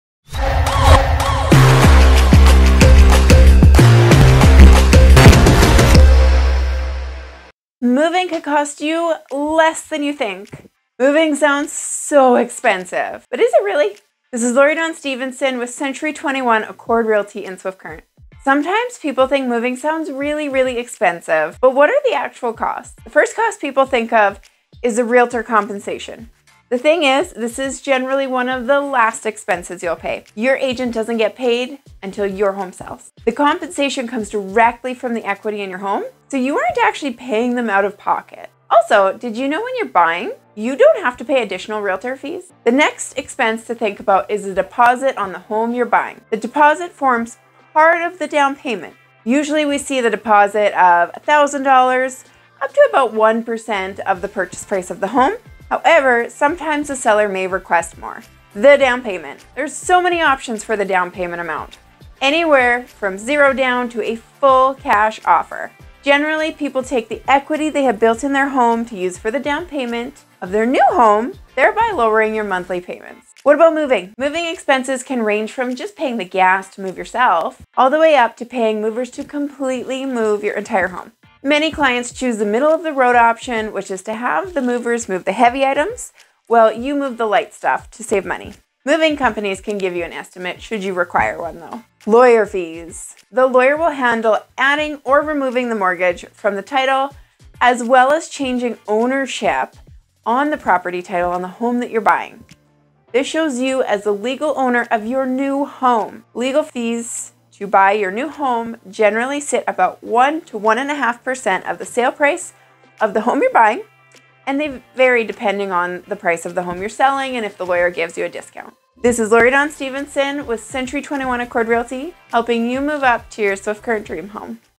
💸 𝐖𝐡𝐚𝐭 𝐚𝐫𝐞 𝐭𝐡𝐞 𝐚𝐜𝐭𝐮𝐚𝐥 𝐜𝐨𝐬𝐭𝐬 𝐨𝐟 𝐦𝐨𝐯𝐢𝐧𝐠? 💸
Lori-Dawn Stevenson, giving you a break down of what 𝑌𝑂𝑈 𝑐𝑎𝑛 𝑒𝑥𝑝𝑒𝑐𝑡 𝑑𝑢𝑟𝑖𝑛𝑔 𝑏𝑢𝑦𝑖𝑛𝑔, 𝑠𝑒𝑙𝑙𝑖𝑛𝑔 𝑎𝑛𝑑 𝑚𝑜𝑣𝑖𝑛𝑔 𝑐𝑜𝑠𝑡𝑠!